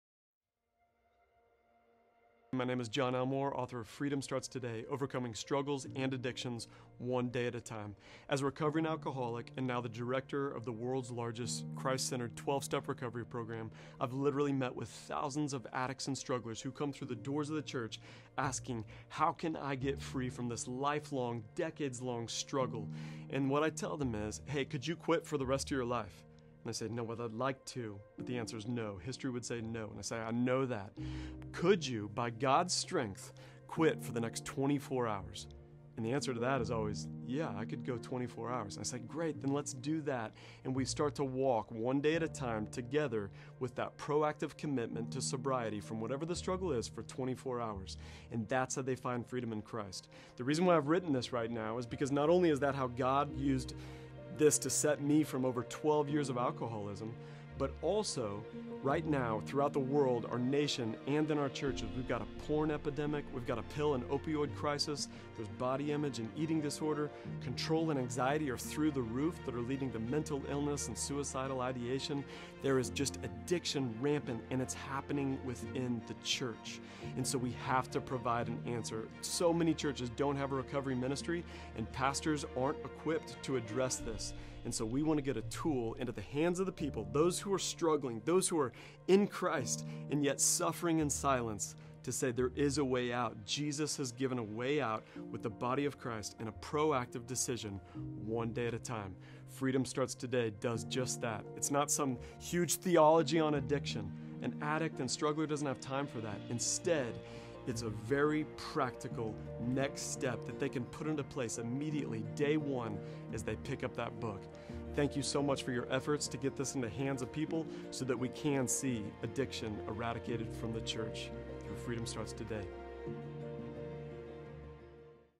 Freedom Starts Today Book Trailer
In Freedom Starts Today, John Elmore makes a huge promise to the addicted: you can be free from your struggle, and much sooner than you may think. Through easily digestible readings grounded in Scripture and the practice of daily surrender, Elmore shows you how to break the cycle of addiction, make war against sin, and find your identity in who you are and not the shame of what you have done--one day at a time.